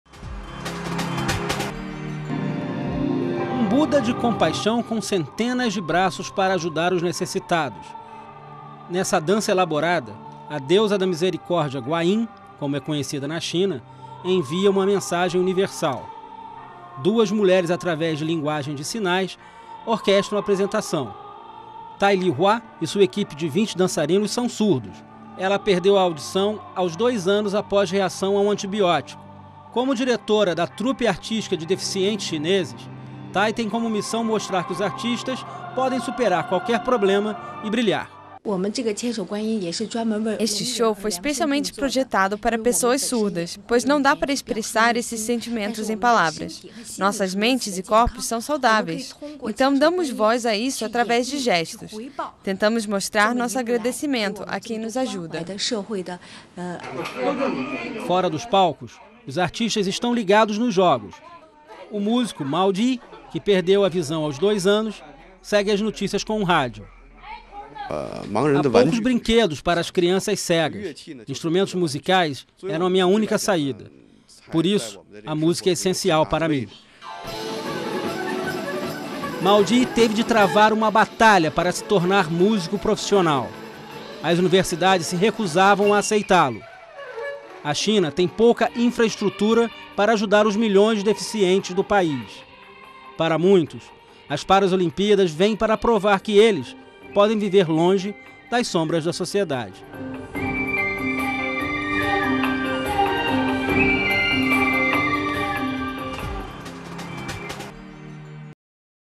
Show de superação
Os artistas cegos e surdos da China têm a chance de brilhar durante as Paraolimpíadas com um show chamado: 'Meu Sonho'... O espírito olímpico de Pequim-2008 ganha o aspecto mágico com uma figura tradicional religiosa.